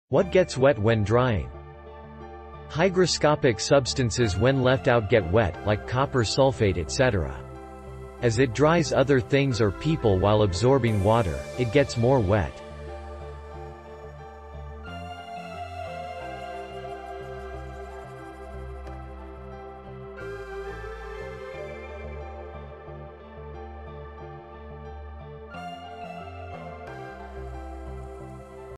What gets wet when drying?
Hygroscopic substances when left out get wet! like copper sulphate etc.!! As it dries other things or people while absorbing water, it gets more wet.